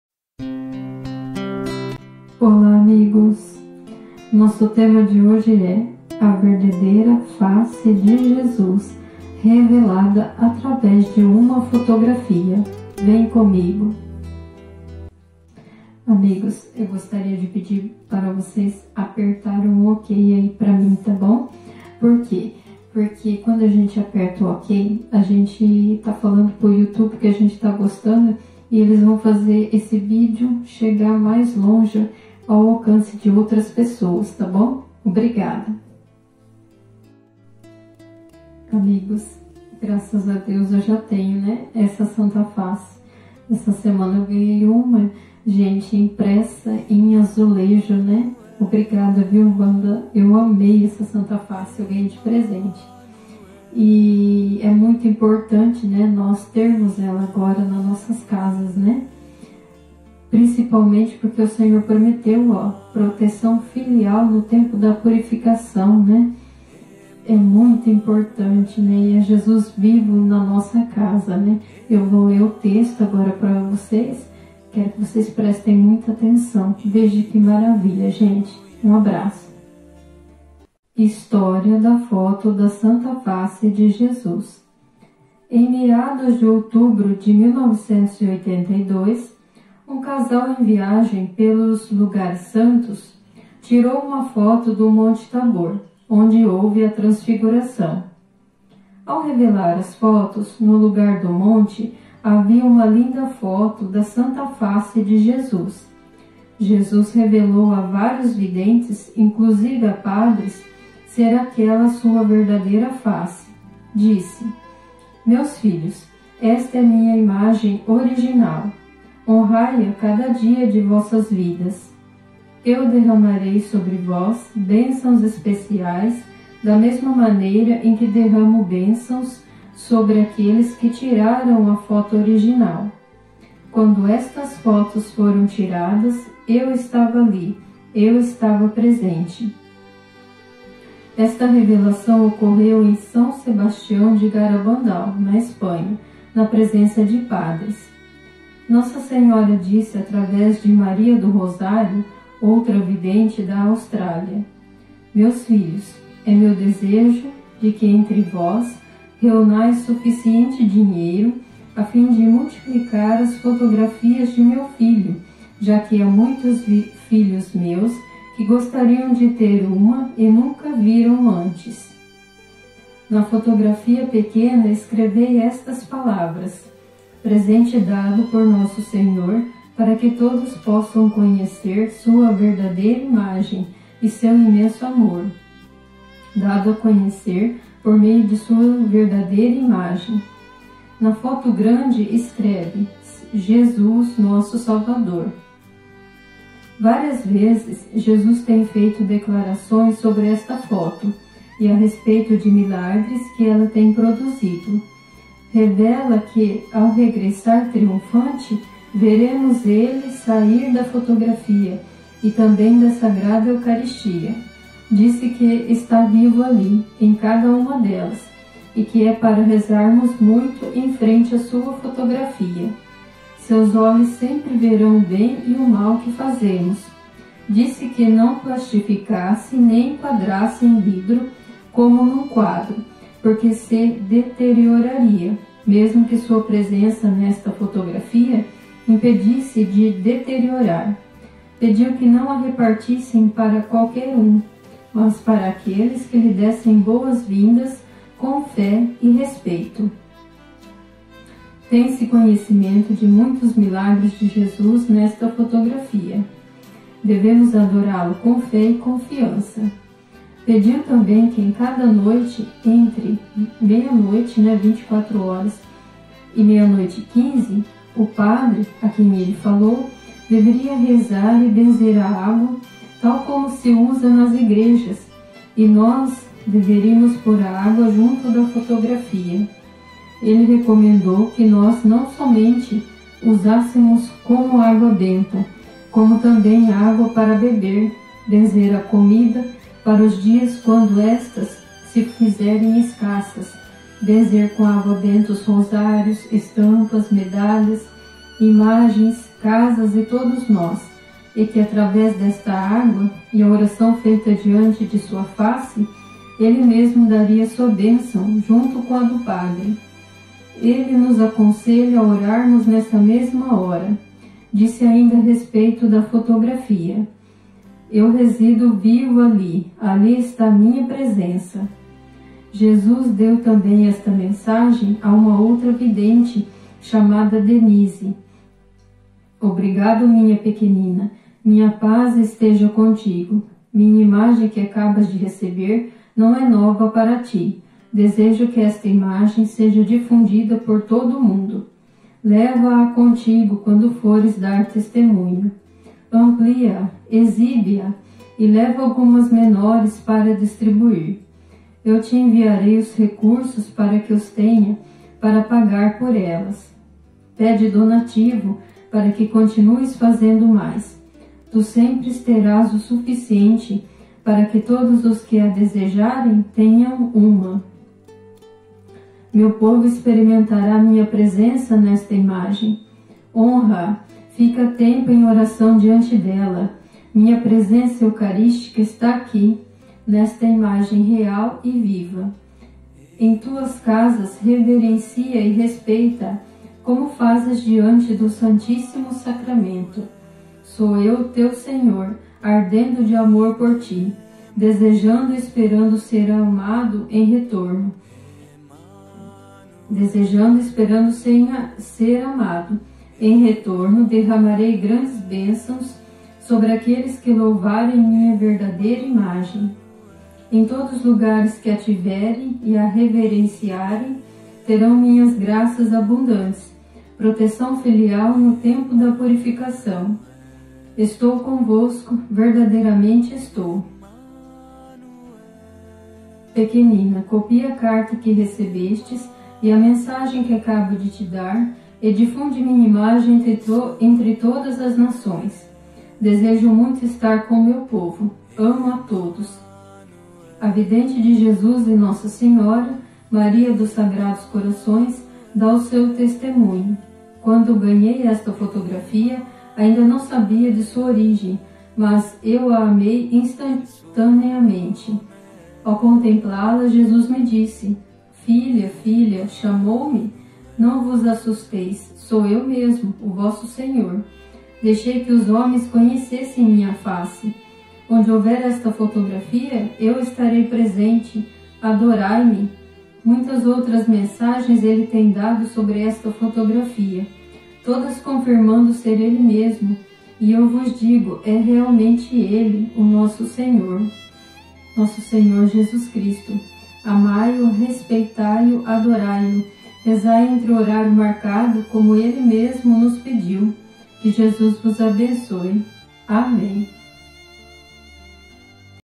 A VERDADEIRA FACE DE JESUS revelada numa fotografia
Proteção para os Tempos Finais! Deus está VIVO nela!Disse que está vivo ali, em cada
uma  delas,  e  que  é  para  rezarmos  muito  em  frente  a
 Sua  fotografia.  Seus  Olhos
sempre  verão  o  bem  e  o  mal  que  fazemos.  Disse  que
 não  plastificasse  nem
enquadrasse  em  vidro  como  num  quadro,  porque  se
  deterioraria,  mesmo  que  Sua
presença nesta fotografia impedisse de deteriorar.
 Pediu que não a repartissem para
qualquer um, mas para aqueles que lhe dessem boas
vindas com fé e respeito.